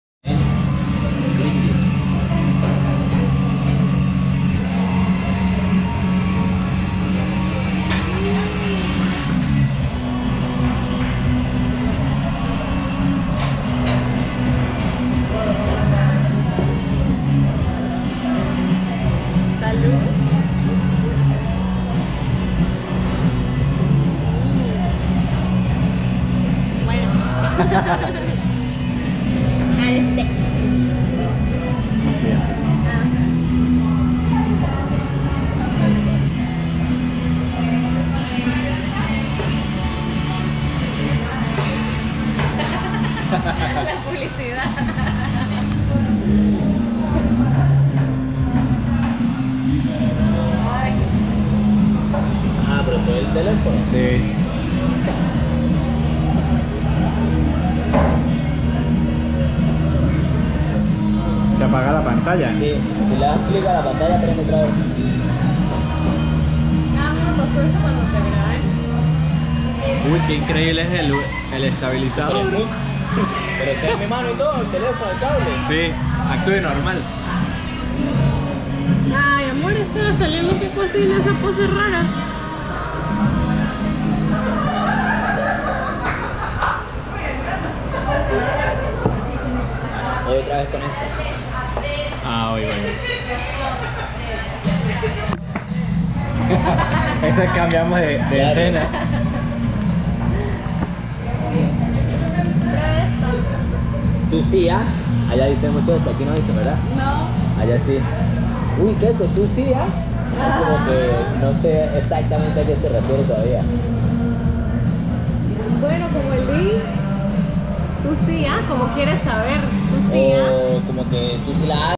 Probando Cerveza Artesanal, en el Taller con Keko y Sarath
Fuimos a probar unas ricas cervezas artesanales en el mejor lugar de la ciudad El Taller, con la excusa de probar la nueva gopro hero7 (GoPro Hero 7).